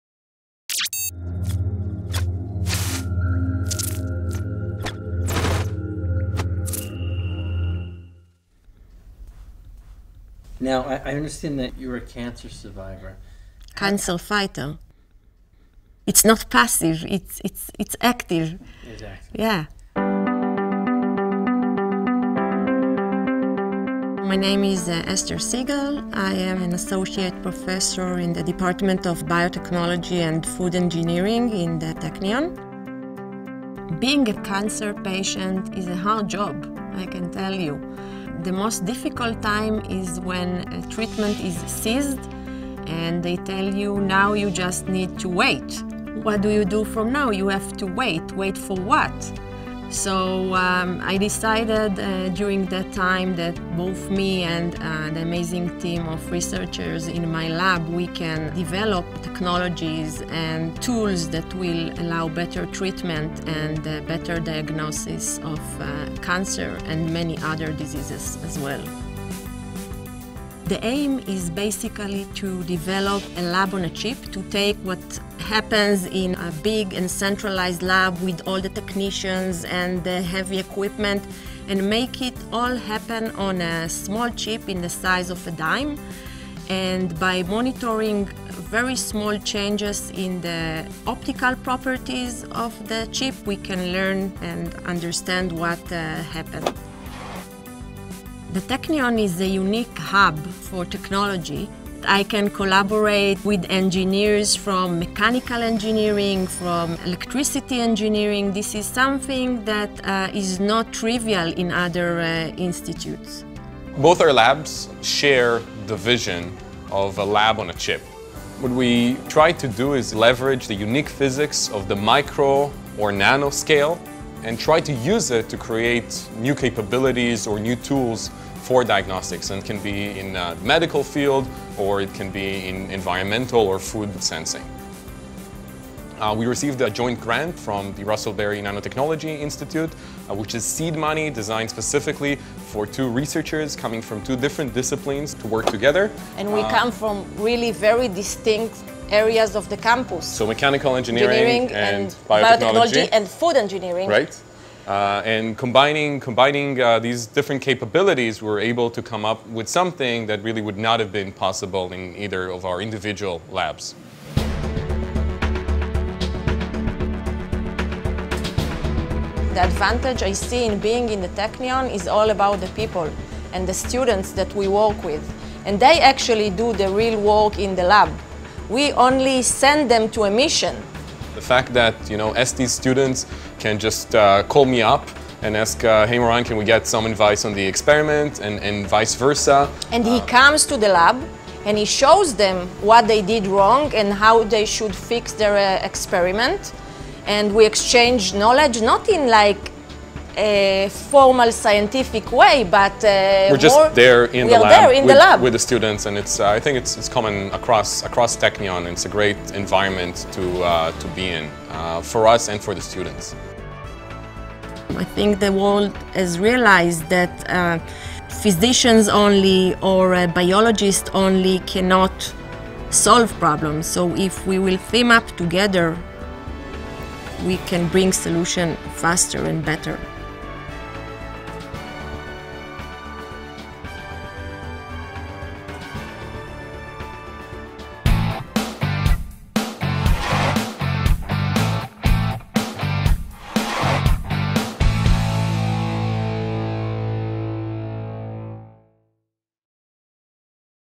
Lab on a Chip Technion Professors Ester Segal & Moran Bercovici
Technion - Israel Institute of Technology Professors Ester Segal of the Faculty of Biotechnology and Food Engineering and Moran Bercovici of the Faculty of Mechanical Engineering are collaborating on a Lab on a Chip.
They are supported by a grant from the Russell Berrie Nanotechnology Institute (RBNI).
Film made by Gilad Tocatly.